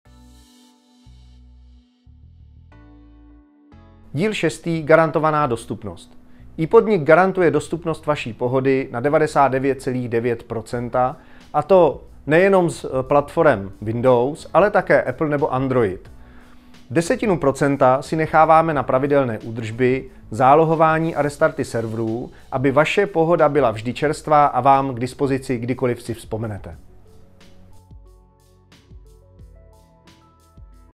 10 Důvodů pro iPodnik - Garantovaná dostupnost
V tomto pokračování našeho seriálu vám představíme 10 důvodů, proč si zvolit iPodnik jako poskytovatele cloudového řešení. V tomto díle vám poskytneme informace o garantované dostupnosti.